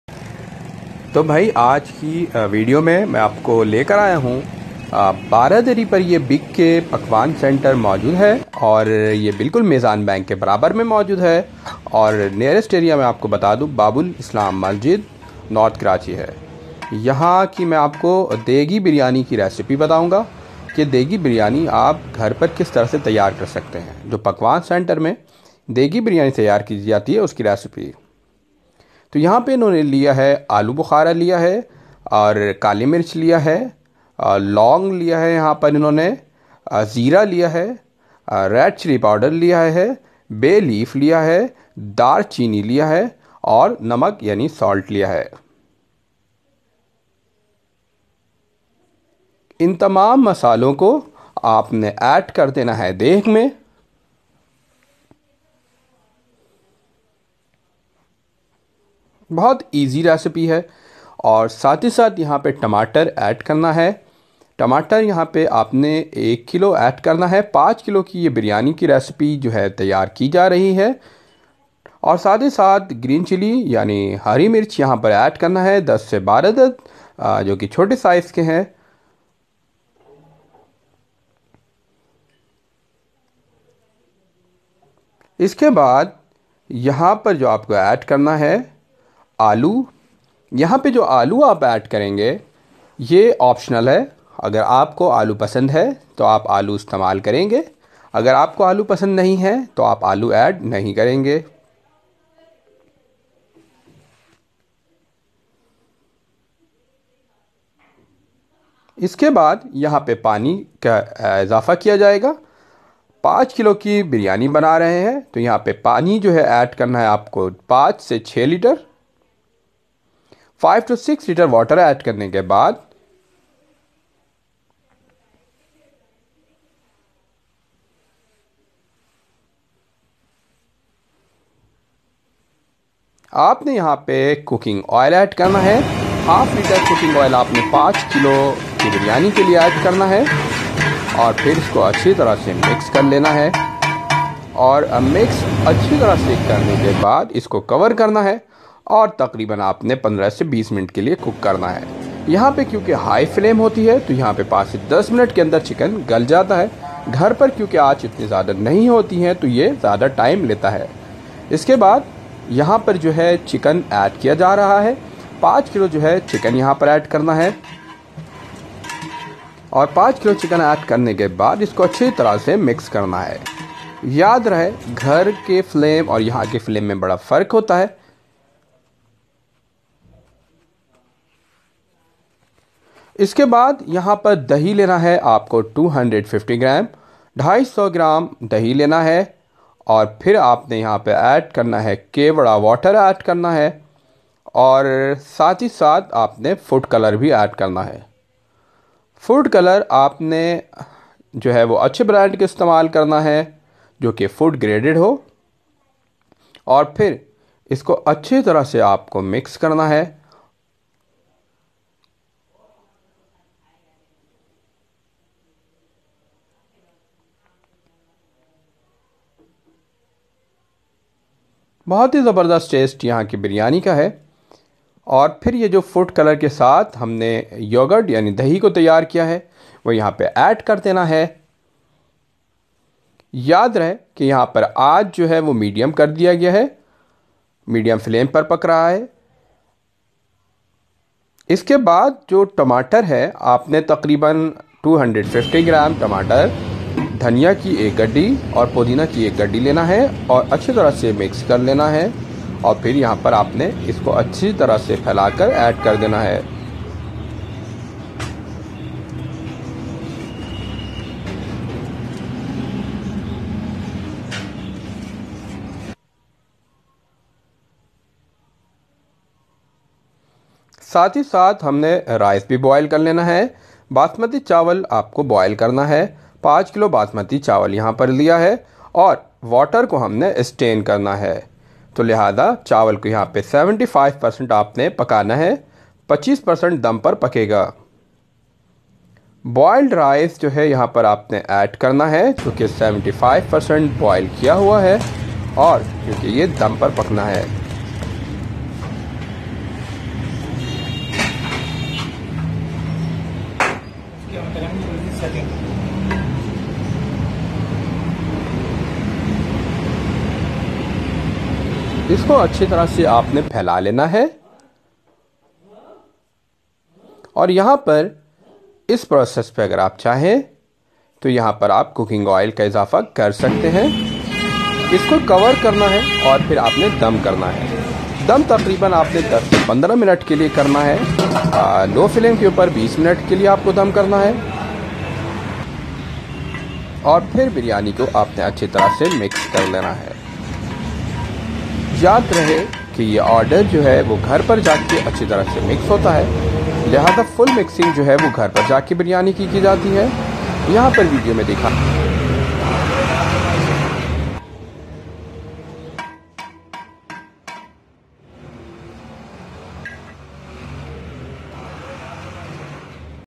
Famous Big-K Degi Biryani Recipe | How To Make Chicken Biryani
Today I am going to disclose famous Big K Degi Chicken biryani recipe